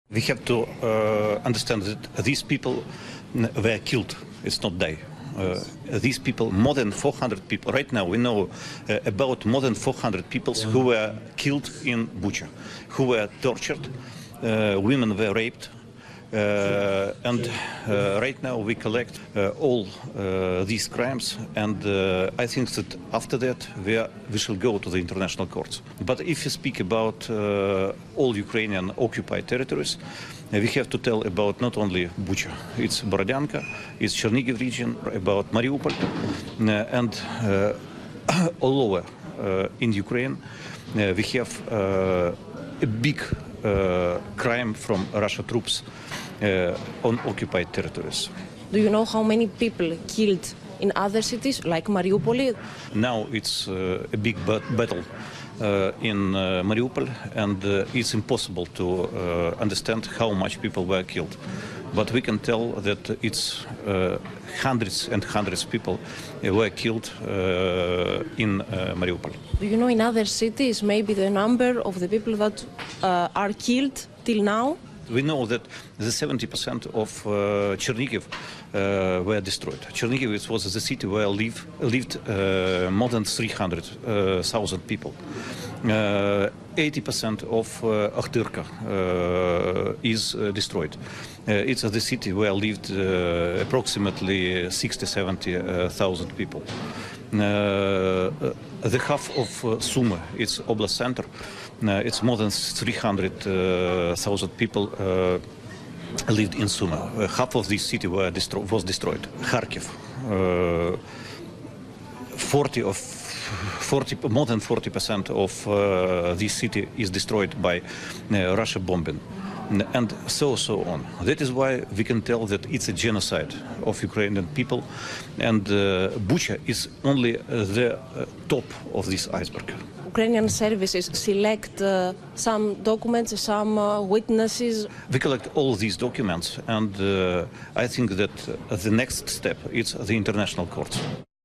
Ο σύμβουλος του υπουργού Εσωτερικών της Ουκρανίας στο OPEN
Γίνετε μέλος στο κανάλι του OPEN για να αποκτήσετε πρόσβαση σε προνόμια:
Ο σύμβουλος του υπουργού Εσωτερικών της Ουκρανίας μίλησε στο OPEN για τις φρικαλεότητες που αποκαλύπτονται για την πόλη Μπούχα, καθώς η γενική εισαγγελία της Ουκρανίας, ανακοίνωσε ότι έχει λάβει περισσότερες από 7.000 αναφορές για εγκλήματα πολέμου.